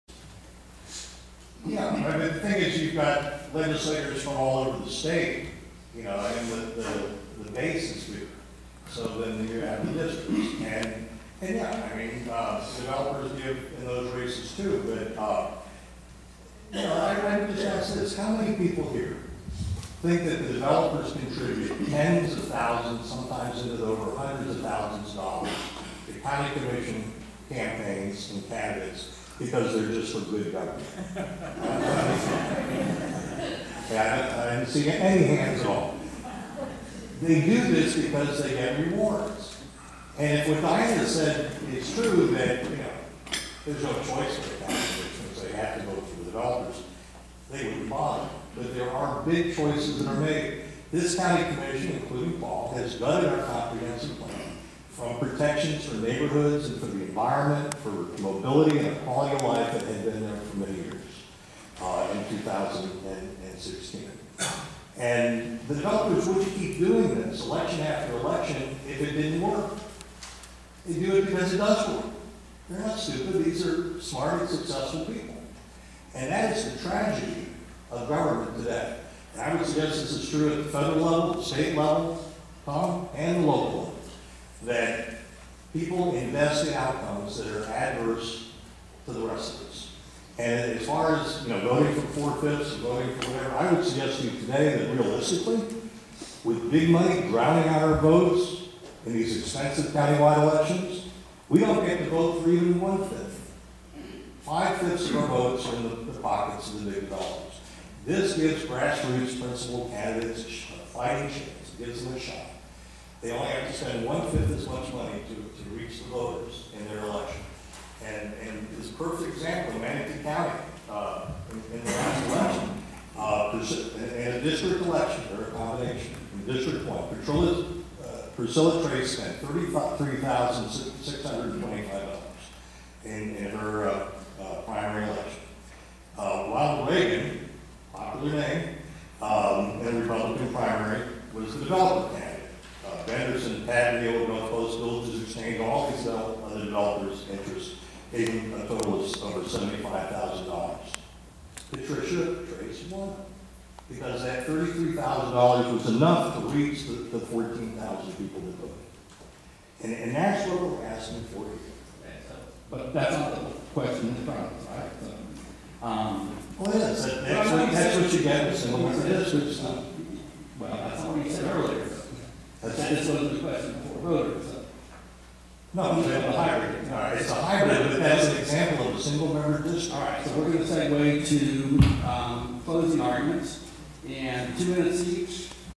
Single Member District Debate - 3
A panel discussion - Dan Lobeck offers his take on why Sarasota needs Single Member District voting.